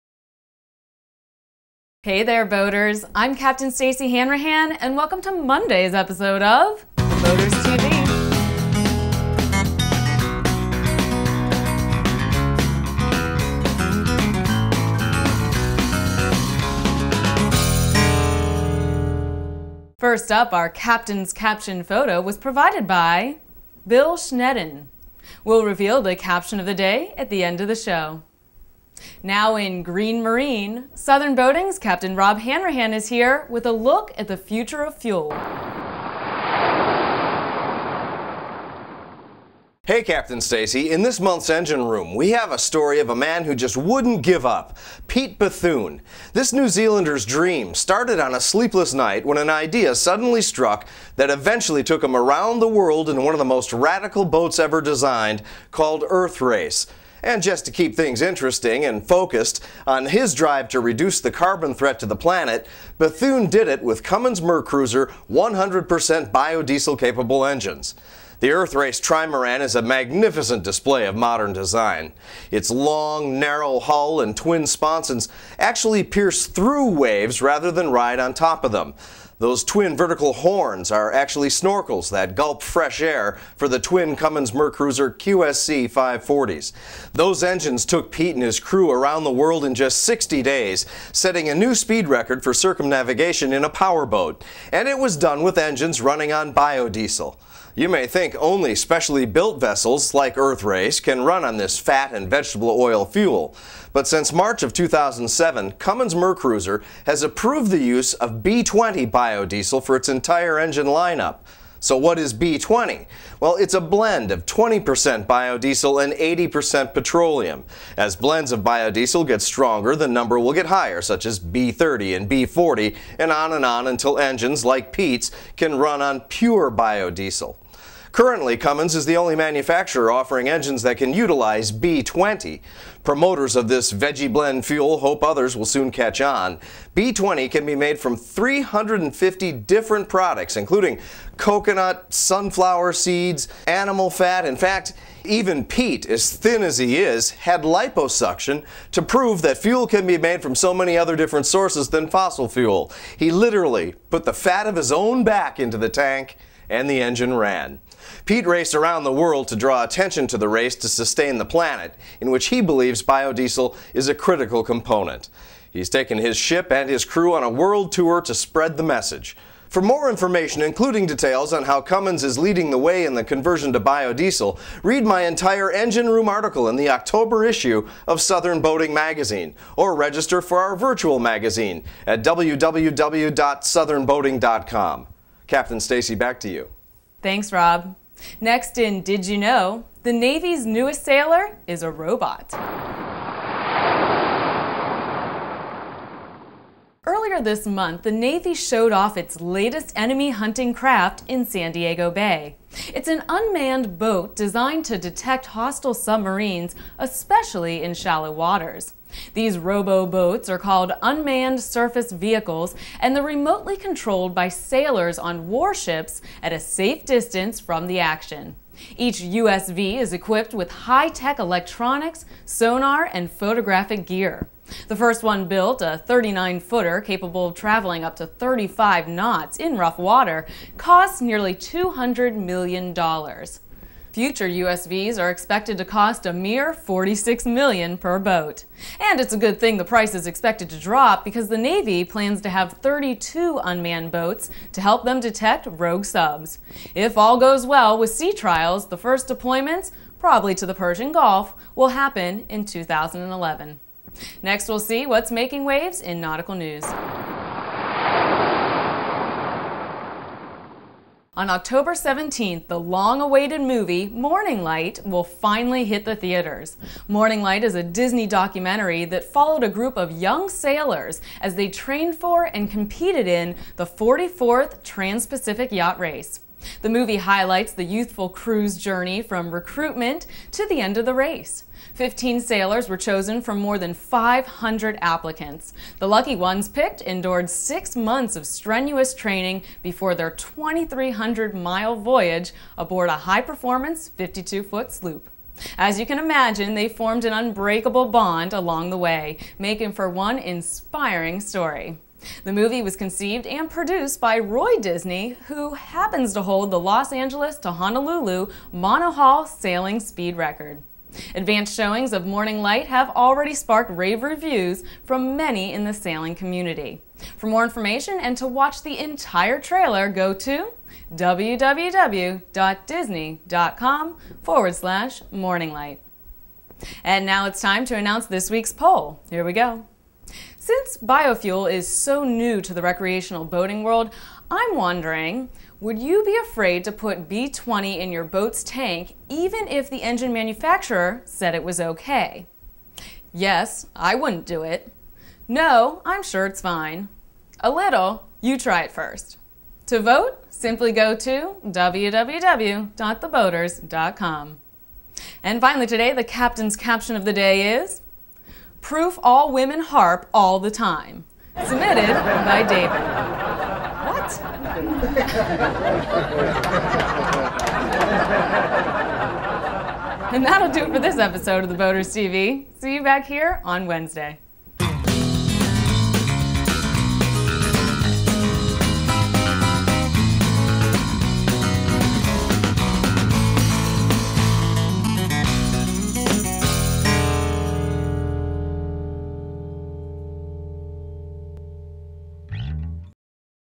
Biodiesel, Unmanned Surface Vehicles, Morning Light
Episode 196
*Southern Boating's Robb Hanrahan is here with the Future of Fuel
*The Navy's Submarine-Seeking Robotic Crafts
*The New Disney Documentary- Morning Light
*Captain's Caption